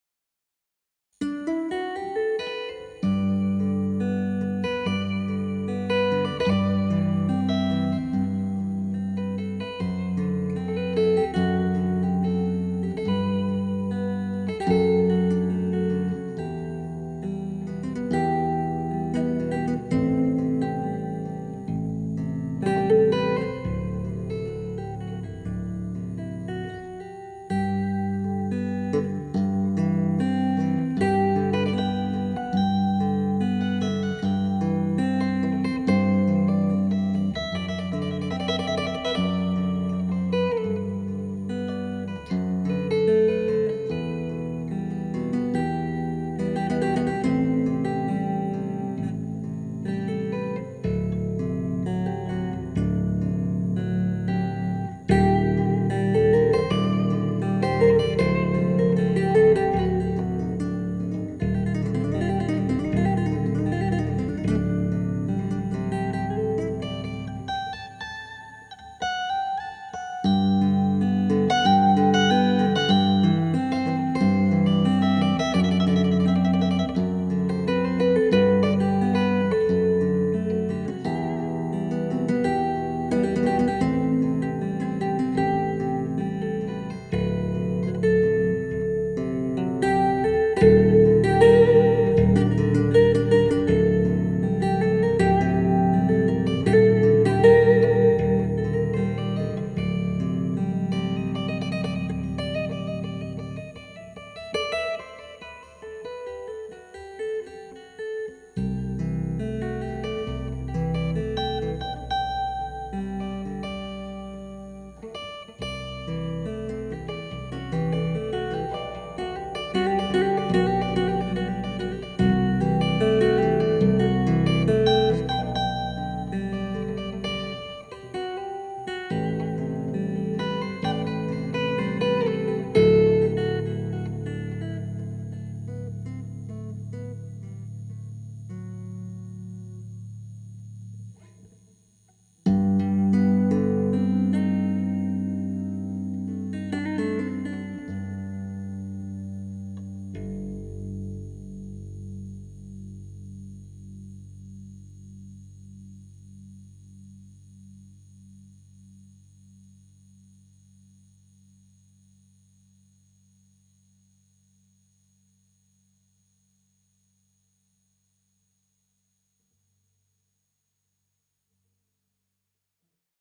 Dedicated to Emmett Chapman,an improvisation on Chapman stick
Dedicated to Emmett Chapman,a little "improvisation" of mine recorded today,sorry for the hissing sound and multiple mistakes during the performance,i haven't played this amazing instrument for way to long,i hope to get my skills back soon
Peace &love to everybody,
Jan